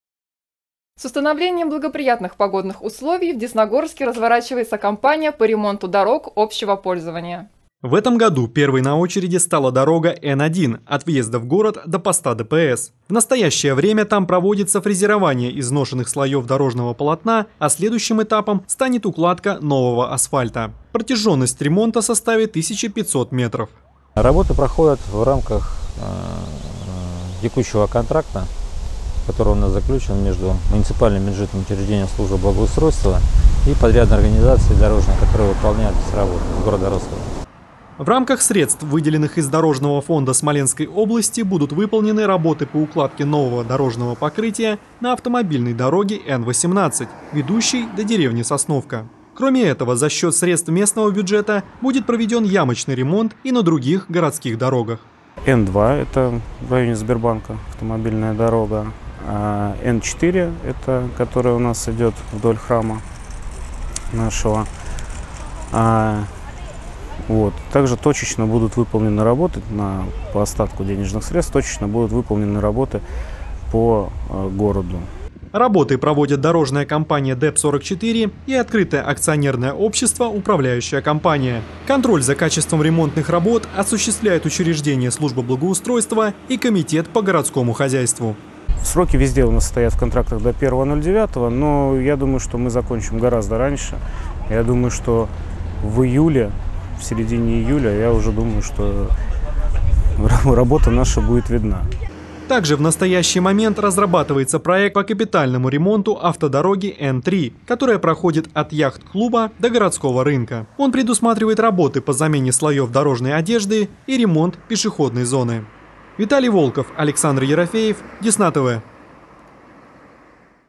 Десна-ТВ: В Десногорске начался ремонт дорог
С установлением благоприятных погодных условий в Десногорске разворачивается кампания по ремонту дорог общего пользования.